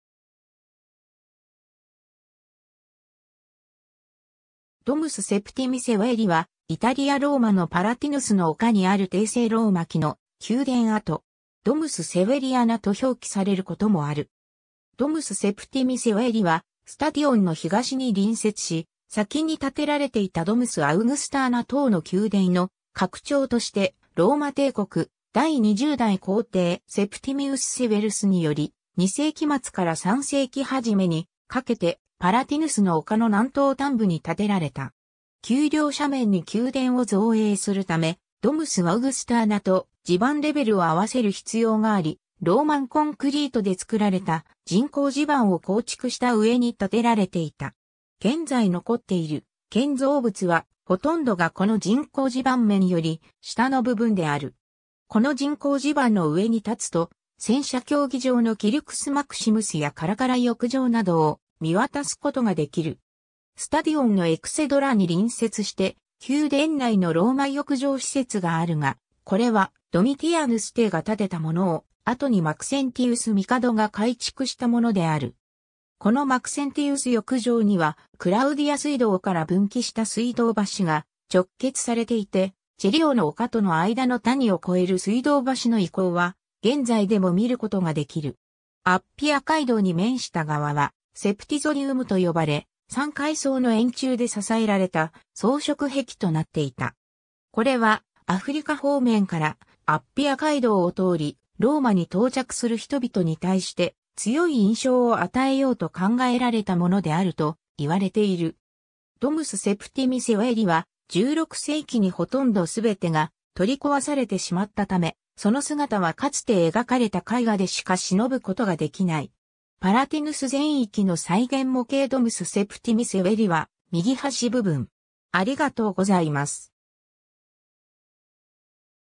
ドムス・セプティミ・セウェリ
セプティミウス・セウェルス
ドムス・セプティミ・セウェリ（ラテン語: Domus Septimii Severi）は、イタリア ローマのパラティヌスの丘（パラティーノの丘）にある帝政ローマ期の宮殿跡。
ドムス・セウェリアナ（Domus Severiana）と表記されることもある。
 ドムス・セプティミ・セウェリはスタディオンの東に隣接し、先に建てられていたドムス・アウグスターナ等の宮殿の拡張として、ローマ帝国第20代皇帝セプティミウス・セウェルスにより、2世紀末から3世紀初めに掛けてパラティヌスの丘の南東端部に建てられた。
 丘陵斜面に宮殿を造営するため、ドムス・アウグスターナと地盤レベルをあわせる必要があり、ローマン・コンクリートで造られた人工地盤を構築した上に建てられていた。
現在残っている建造物は、ほとんどがこの人工地盤面より下の部分である。
この人工地盤の上に立つと、戦車競技場のキルクス・マクシムスやカラカラ浴場などを見渡すことができる。
 スタディオンのエクセドラに隣接して宮殿内のローマ浴場施設（マクセンティウス浴場）があるが、これはドミティアヌス帝が建てたものを、後にマクセンティウス帝が改築したものである。
このマクセンティウス浴場には、クラウディア水道から分岐した水道橋が直結されていて、チェリオの丘との間の谷（現在のVia di San Gregorio）を越える水道橋の遺構は現在でも見ることができる。
 アッピア街道（現在のVia di San Gregorio）に面した側はセプティゾディウム（ラテン語: Septizodium）と呼ばれ、3階層の円柱で支えられた装飾壁となっていた。
これは、アフリカ方面からアッピア街道を通りローマに到着する人々に対して、強い印象を与えようと考えられたものであると言われている。
 ドムス・セプティミ・セウェリは、16世紀に（人工地盤の構造物を除いて）ほとんどすべてが取り壊されてしまったため、その姿はかつて描かれた絵画でしか偲ぶことができない。
 パラティヌス全域の再現模型 ドムス・セプティミ・セウェリは右端部分